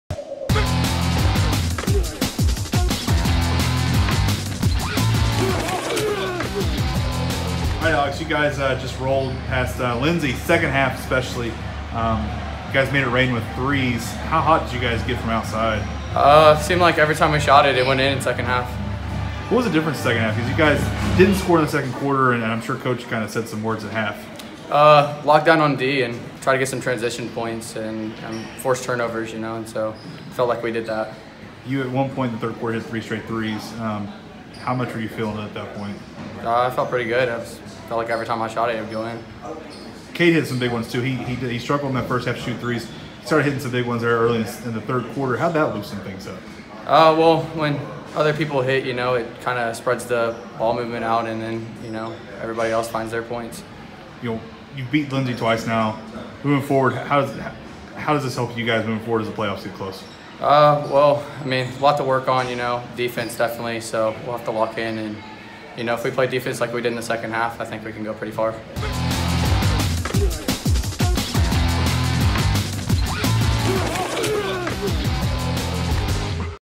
Varsity: Washington boys beat Lindsay
Washington’s Alex Foster talks about the 62-37 win over Lindsay.